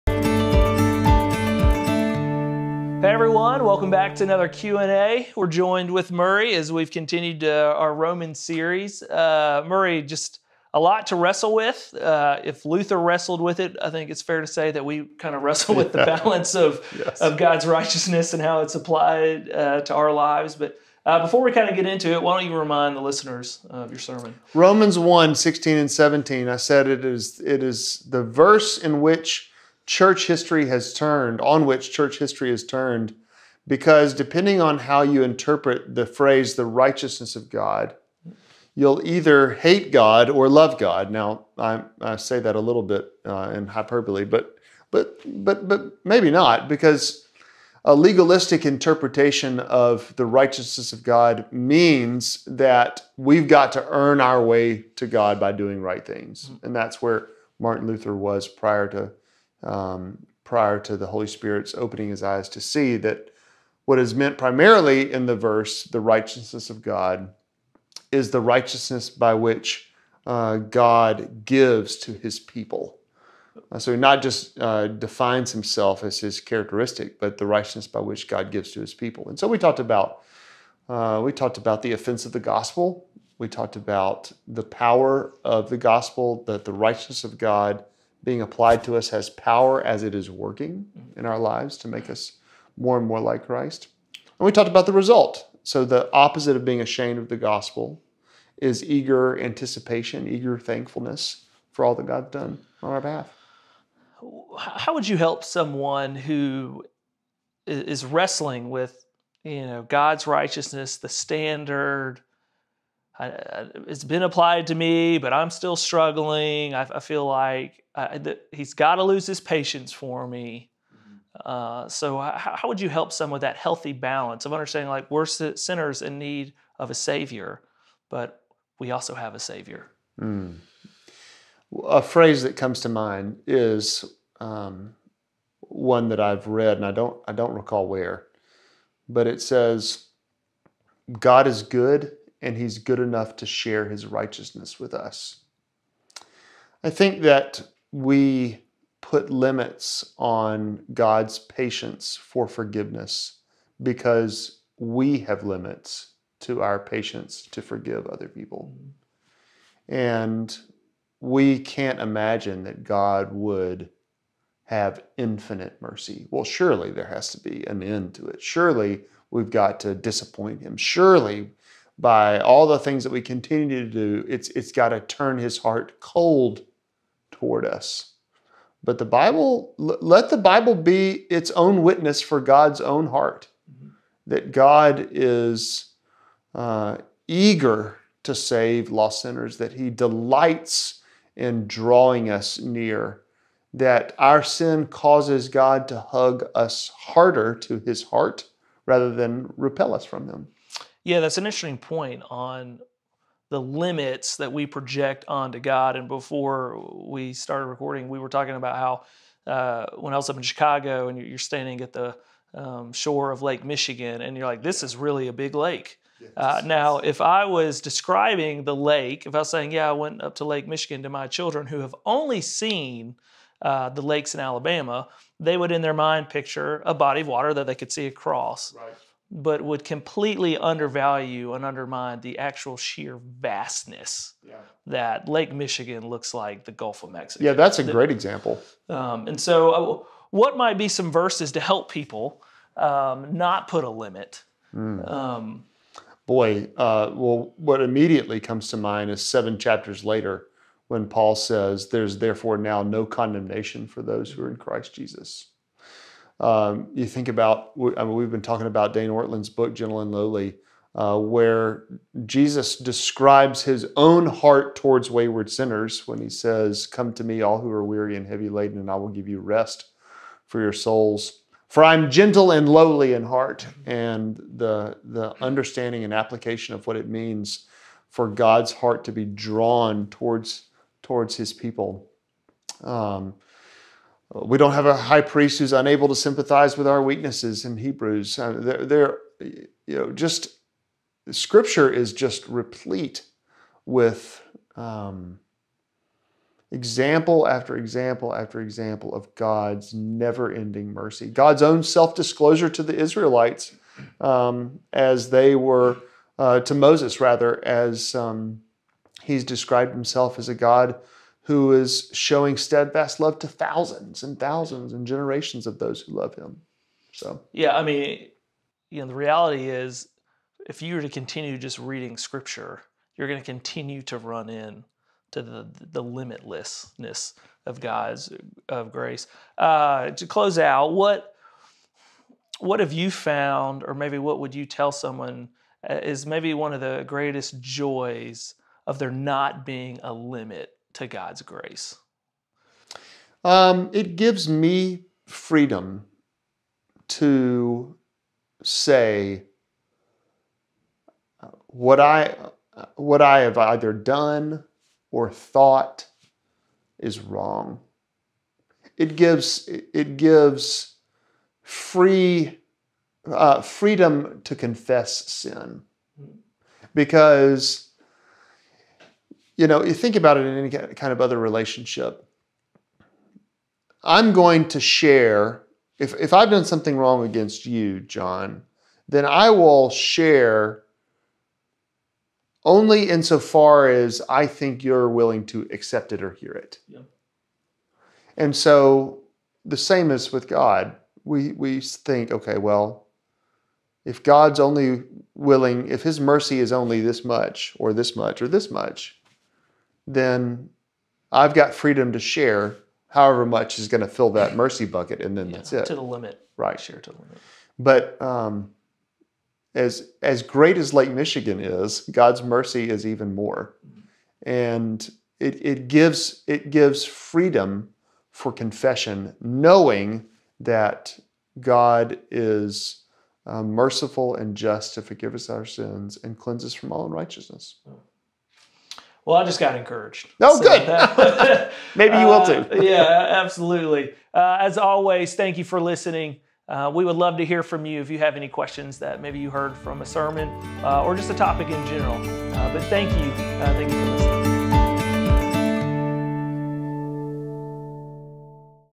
Sermon Q & A - September 27, 2021
Murray and John discuss some topics related to Sunday's sermon (September 26, 2021).